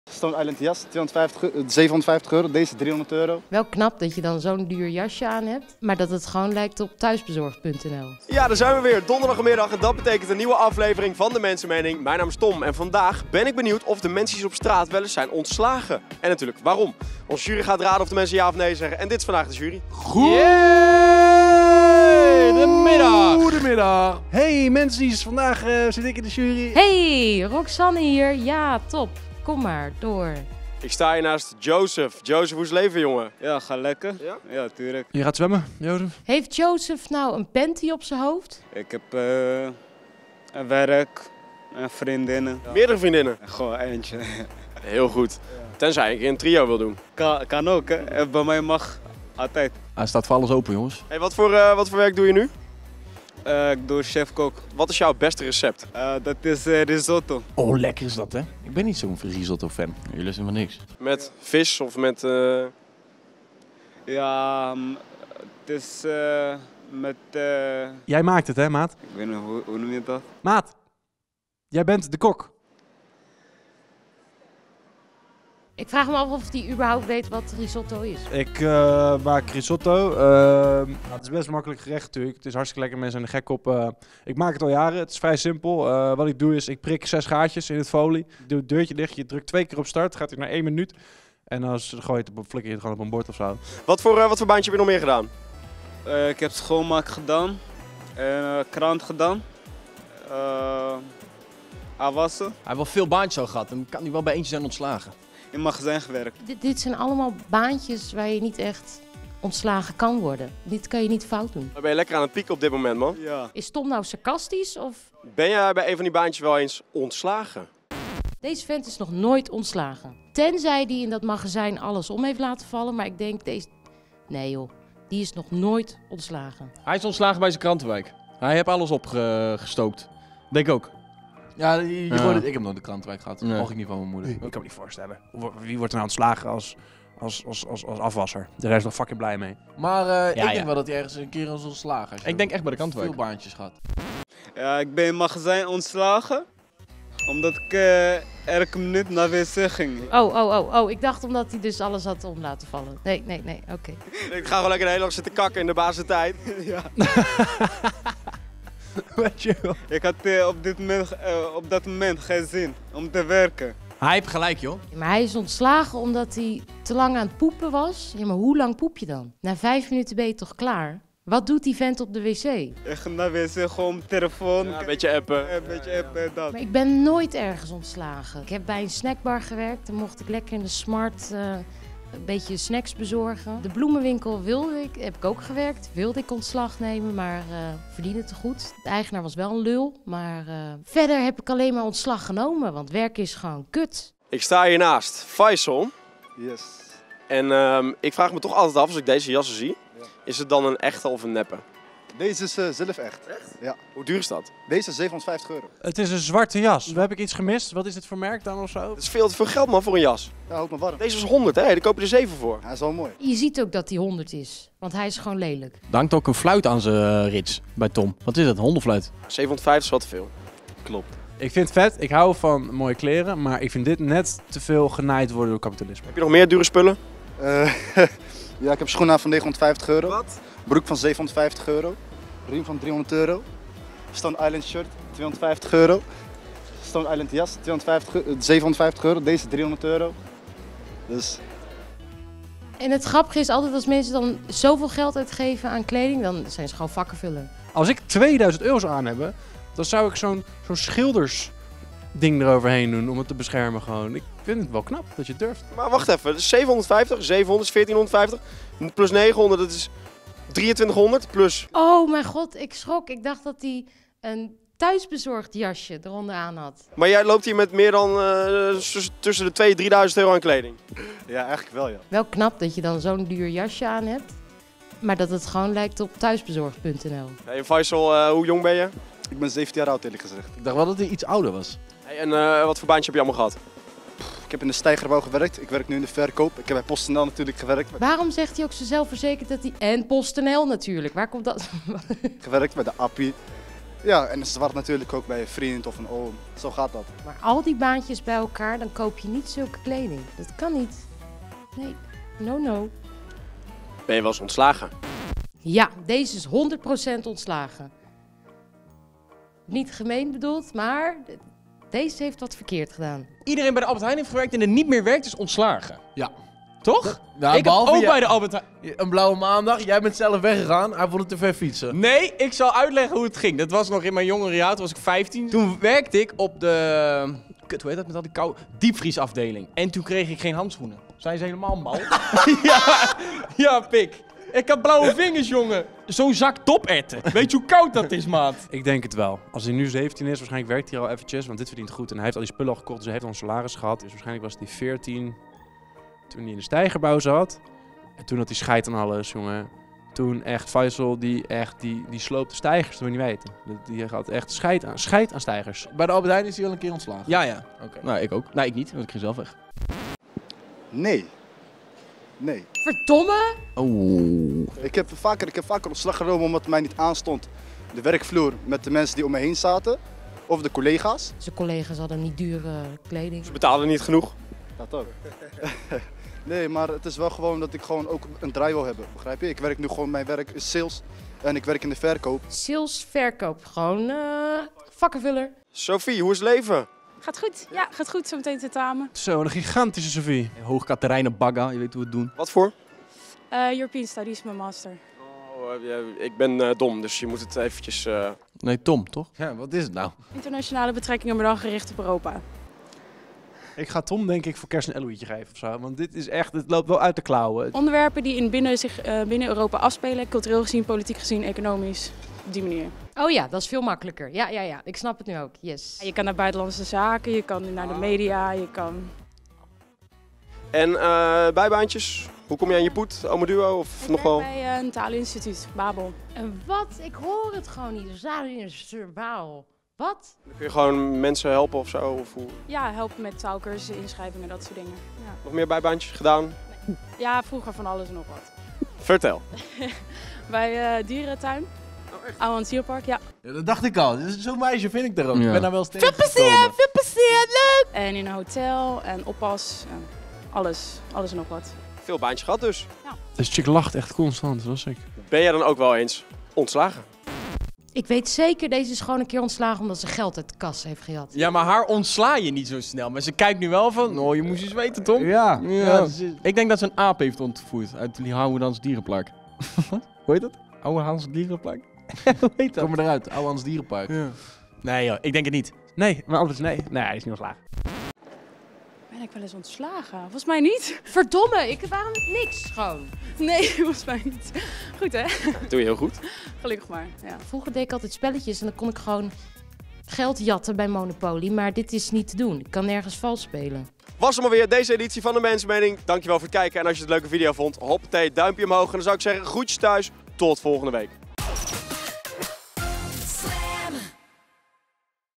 'Jij loopt in kleding van €3000,-' - De Mensenmening | SLAM!
Tom van der Weerd gaat elke week met een vraag de straat op. De jury geeft vervolgens hun ongezouten mening over alles wat mensen daarop antwoorden. Deze week is de vraag: Ben je ooit ontslagen?

Meer zien van Tom?

Meer van de jury?

Play Music, Play Life.

SLAM!